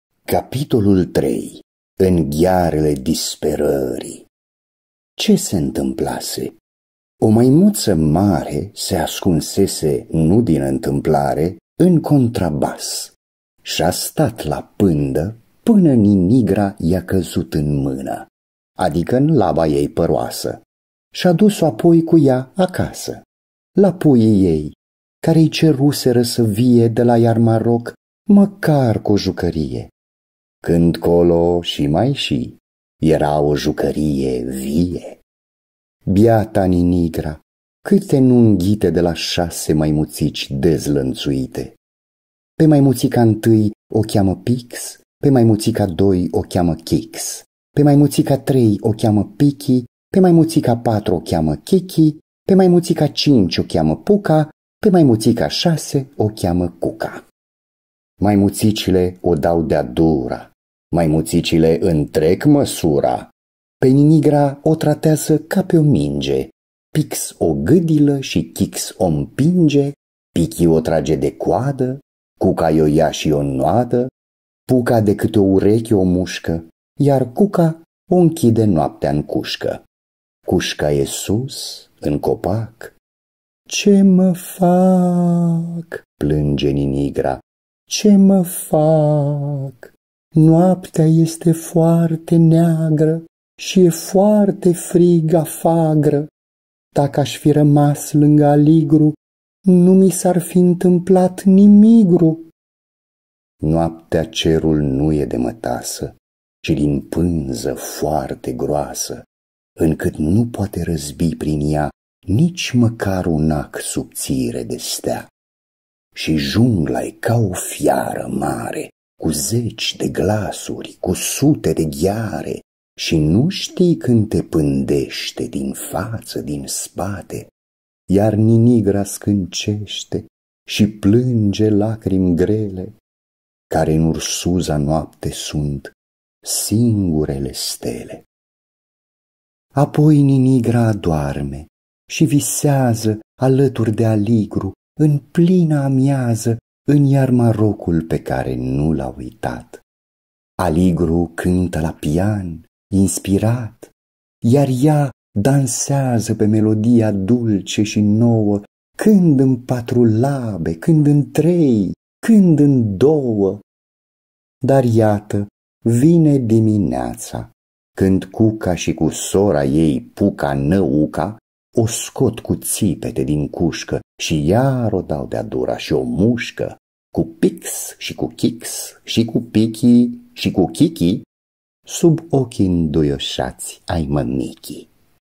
Povestea a doi pui de tigru, numiti Ninigra si Aligru, Capitolul 3
Capitolul 3: In ghearele disperarii
de Nina Cassian
ilustratia: Karda Zenko
lectura: Damian Victor Oancea
editura Frontiera